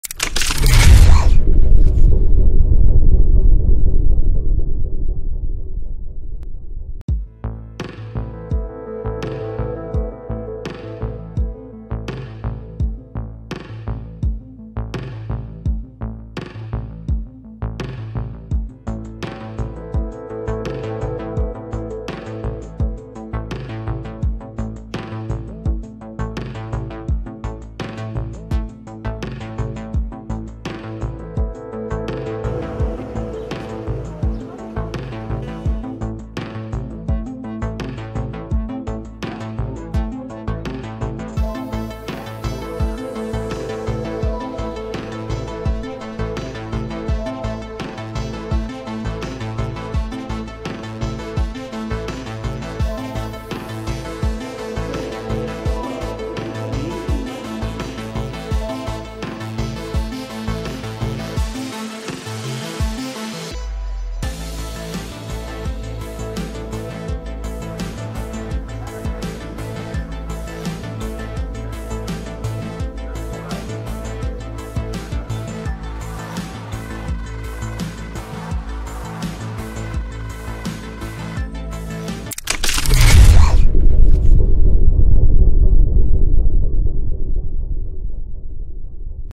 Historic Mosque turned into Nightclub
Palestinian Historical Mosque Changed to a Nightclub by the israelis